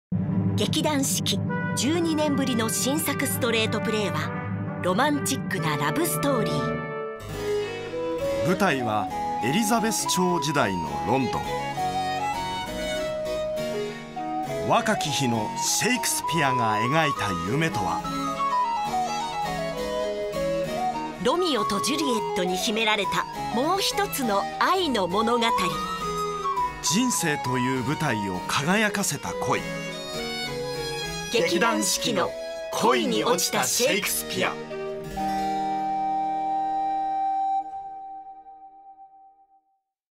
劇団四季：恋におちたシェイクスピア：プロモーションVTR
劇団四季の新作ストレートプレイ『恋に落ちたシェイクスピア』のプロモーションVTRです。
◆公演情報
2018年6月、10月東京公演（自由劇場）、同9月京都公演（京都劇場）、同12月福岡公演（キャナルシティ劇場）